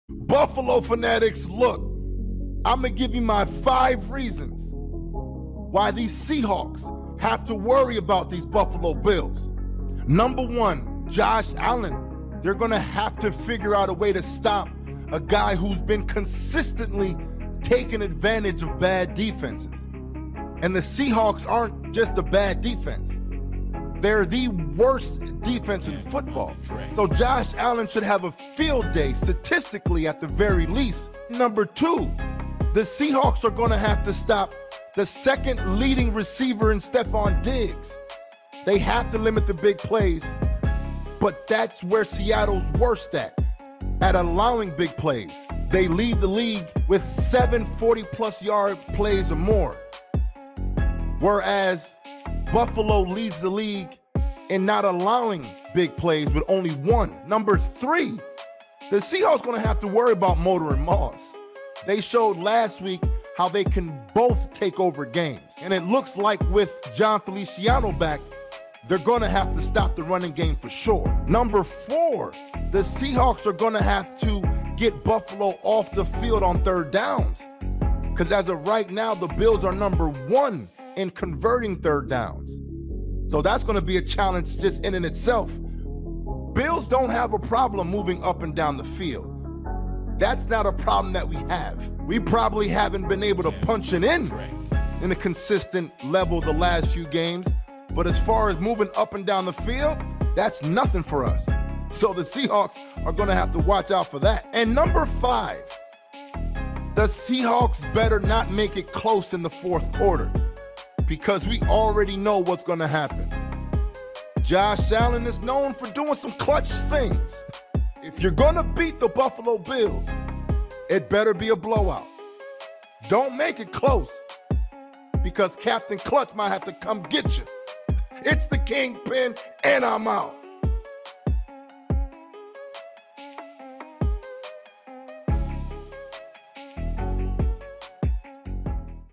5 Reasons WHY Seahawks have to WORRY about the Buffalo Bills
So much talk is about how the Buffalo Bills can win. The Kingpin decides to make a video on what the Seahawks have to be careful of when facing the Bills. 5 reasons why Seahawks have to worry about the Buffalo Bills

Buffalo Bills Buffalo Fanatics Bills